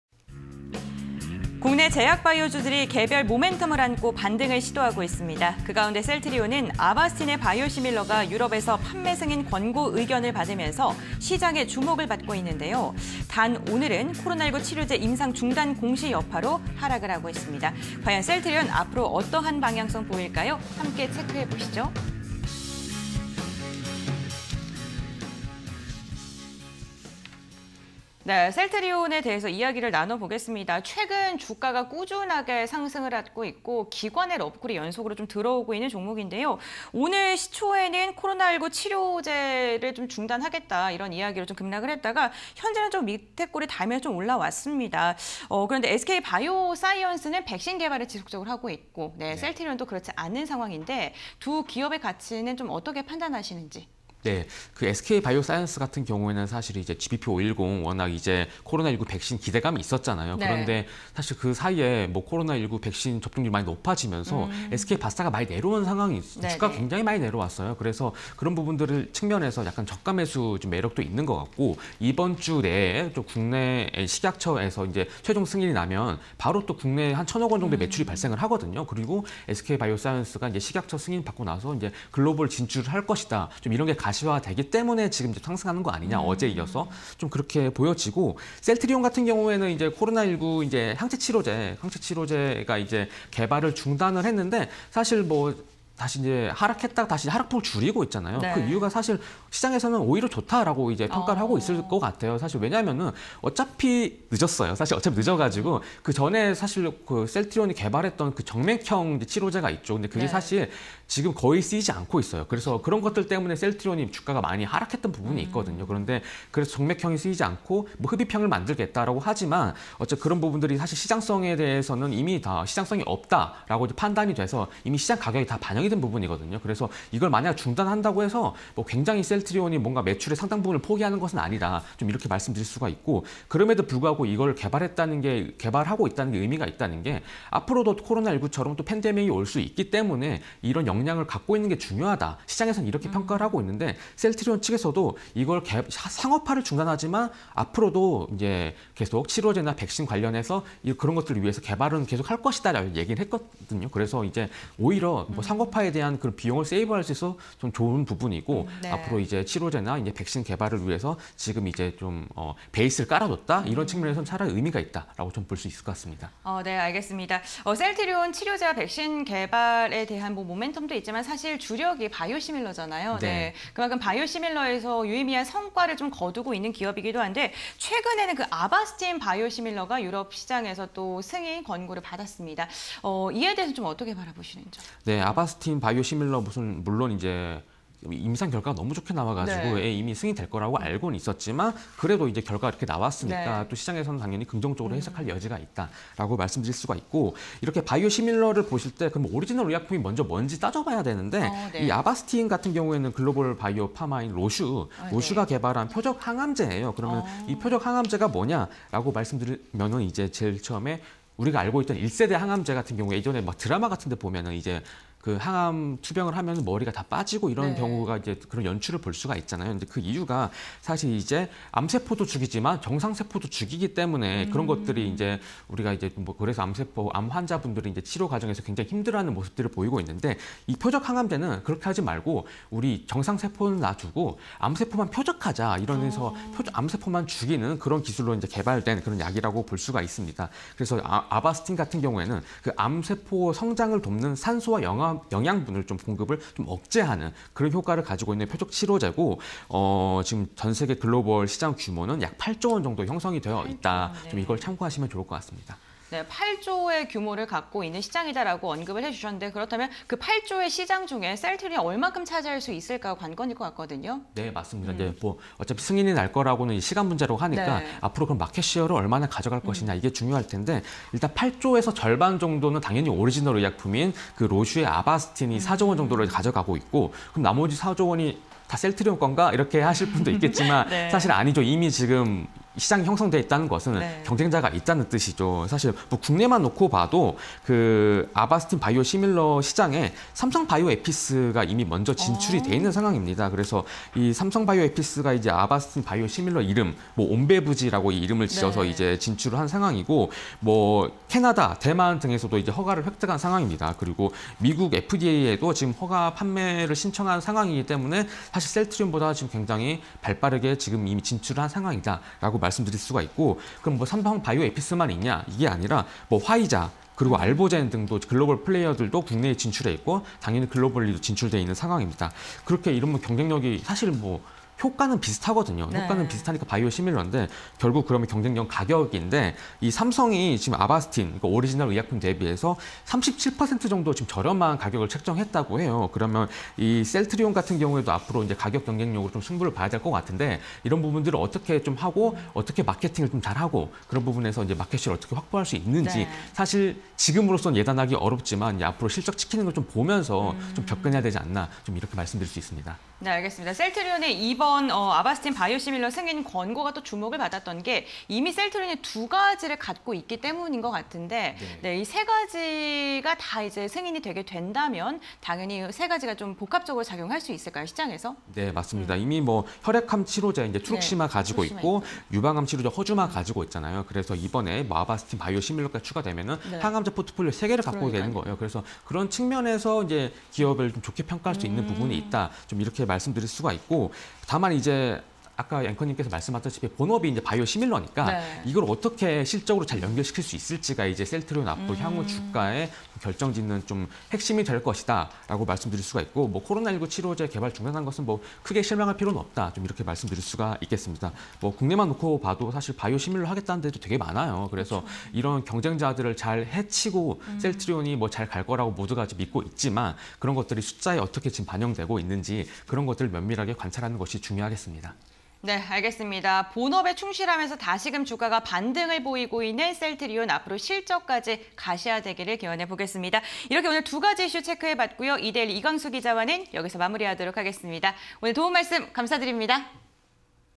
셀트리온 '아바스틴 바이오시밀러' 유럽 진출 '눈앞'_이슈 파워 (20220628)
Q. 코로나19 치료제 성과에 SK바이오↑ VS 셀트리온↓ 이유는?
- 국산 코로나 백신·치료제 개발 막바지, 기업 성과 희미 엇갈려
- SK바이오 '스카이코비원멀티주' 백신 중앙약사심의위원회 승인
- SK바이오, 코로나 백신 관련 투자자 기대감 증폭
- 셀트리온, 코로나19 흡입형 항치료제 임상시험 중단
- 복제약 승인 등 탄력받은 셀트리온 주가 하락

Q. '셀트리온' 유럽 판로 열리나, 글로벌 진출 전망은?
- 바이오시밀러 'CT-P16' 유럽 판매 승인 권고

Q. '셀트리온' 글로벌 항암제 시장 선도 가능성은?
- '로슈' 아바스틴으로 글로벌 매출 4조 원 기록
- 아바스틴 바이오시밀러 시장 '삼성바이오에피스' 선점
- 유럽 판매 중·美 판매 허가 신청 등 시장 진출
- '삼성바이오에피스' 오리지널 比 37% 저렴한 판매가
- 국내외 경쟁사 다수, 시장 점유율 확보 여부 관심

Q. 유럽의약품청 '판매 승인 권고' 의미는?
- 유럽의약품청 CHMP '판매 승인 권고' 의견
- 최종 허가 시 셀트리온 항암제 포트폴리오 3개 확보
- 바이오시밀러 시장 경쟁 치열… 수익률 확보 대책 필요
- 국내 제약바이오 기업, 바이오시밀러 상업화 추진
- 진입 기업 증가, 선점 효과에도 마진 불확실성 키워